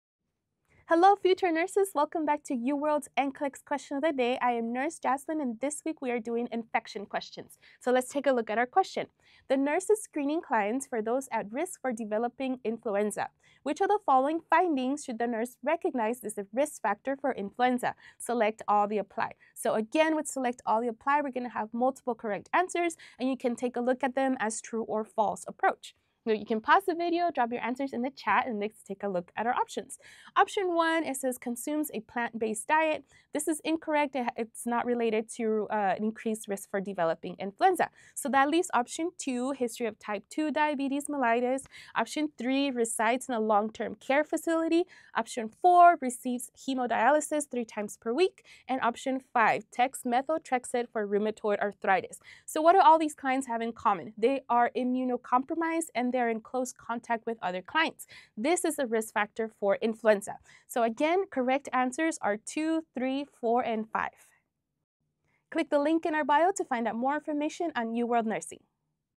QOTD Infection Week
It’s time for our NCLEX® Question of the Day! Nurse Jazmine will be covering influenza! Share and save to add to your NCLEX® study resources.